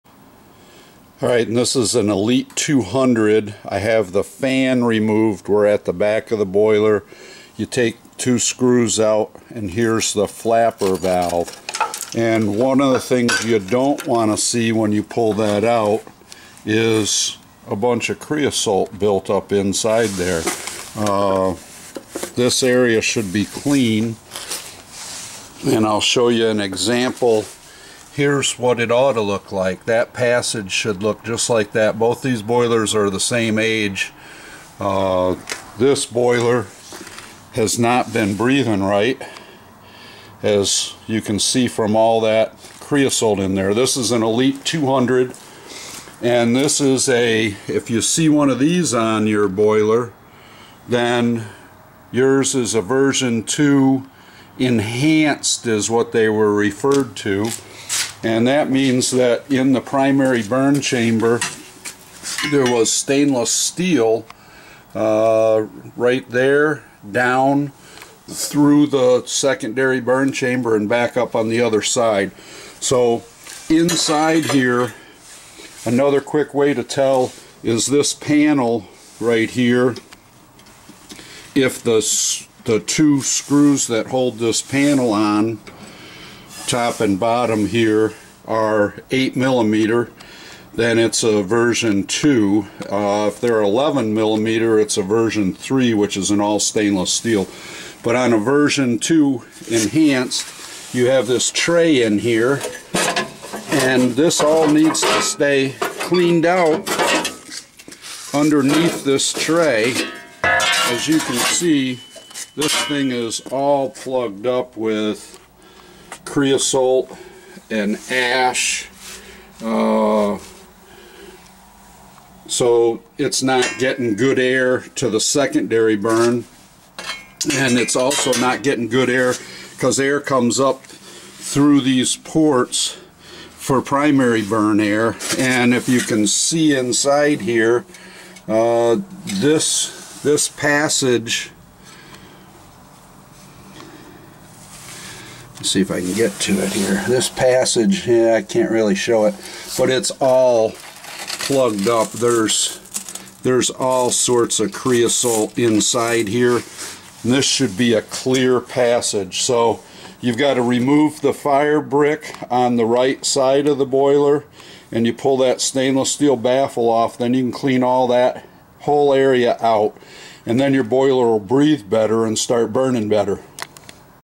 Elite 200 V2 air passage cleanout
Empyre Elite 200 indoor wood gasification boiler not running correctly and creosote is building up behind blower fan and in the air passages. Here is how you fix the problem.